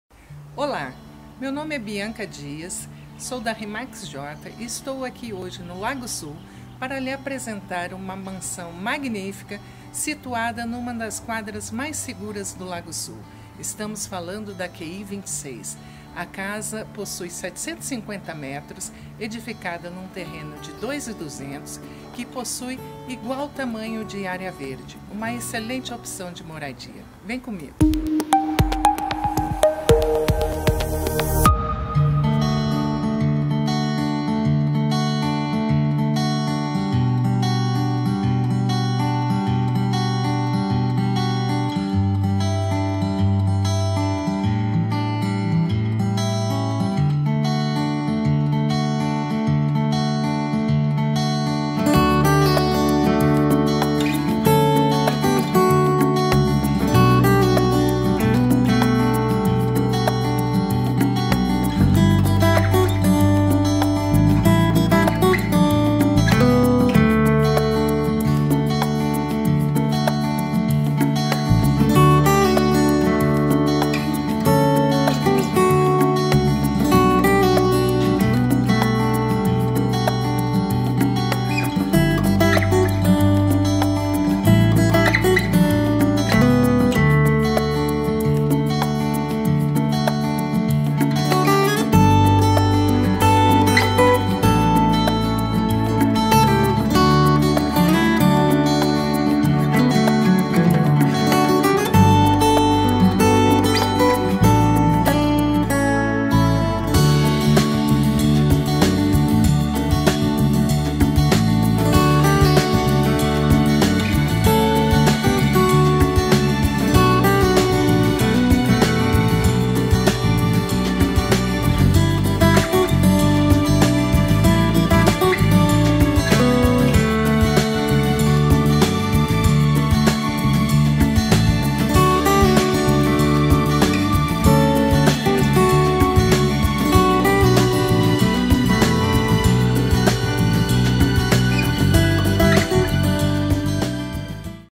MELHOR CASA DA QI 26 NO LAGO SUL - BIANKA DIAS - RE/MAX JOTA
Para você que procura tranquilidade, bom gosto, espaço e muita segurança, encontrou o imóvel certo.

A presente casa está localizada em área nobre de Brasília, edificada em terreno de 2.100 m² + 2000 m² de área verde e possui 750 m² de área privativa. A QI 26 é uma das quadras mais seguras do Lago Sul em função de acolher uma das residências oficiais da Embaixada Americana em Brasília.

Projeto assinado pela arquiteta Ana Paula Barros; a casa é a tradução de requinte e bom gosto. Muito iluminada, arejada e aconchegante, a casa foi construída em dois pavimentos, sendo:

TÉRREO: Hall de entrada, sala de estar com 2 ambientes, sala de jantar, cozinha gourmet do casal integrada com estar íntimo, belíssima adega para 600 garrafas, lavabo, uma suíte que integra com uma ampla varanda, cozinha, 2 despensas, 2 DCE, lavanderia com área verde, garagem para 8 carros. As salas possuem pé direito duplo.
1º ANDAR: excelente área de circulação, sala de TV, 2 suítes, uma suíte máster com 2 closets, 2 banheiros e uma ampla varanda com vista para o lago, sala fitness.

LAZER: Ambiente amplo para seus eventos, sauna úmida, duas piscinas com aquecimento solar, jardins maravilhosos.

Condomínio fechado, com sistema de câmeras de segurança, alarme e toda infraestrutura.

Caso haja interesse, temos várias possibilidades para negociar e viabilizar sua aquisição.